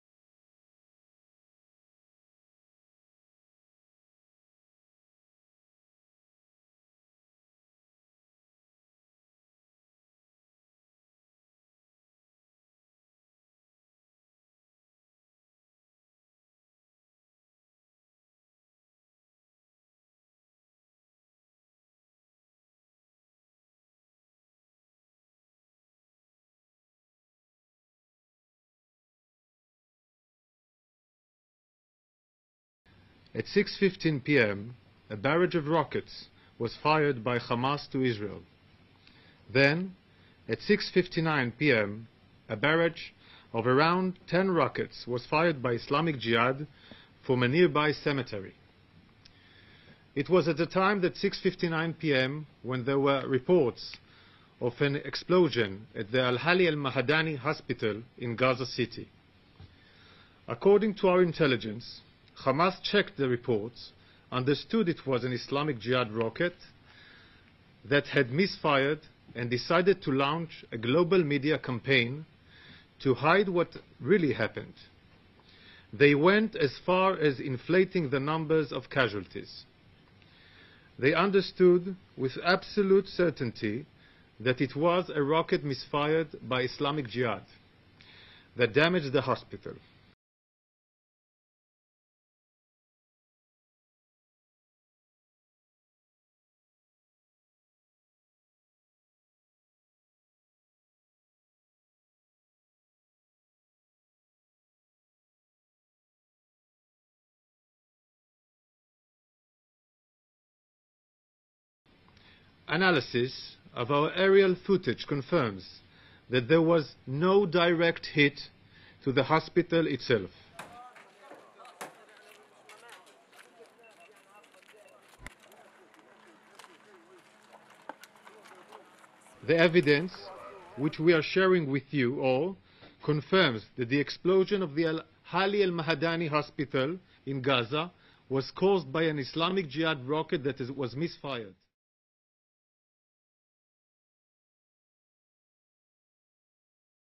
IDF release footage to 'prove' it's not responsible for Gaza hospital strikes
Palestinian officials blamed an Israeli air strike for the huge blast and fireball which engulfed the Al-Ahli al-Arabi hospital. Israel denied responsibility and said the blast was caused by a failed rocket launch by the Palestinian Islamic Jihad militant group, which denied blame.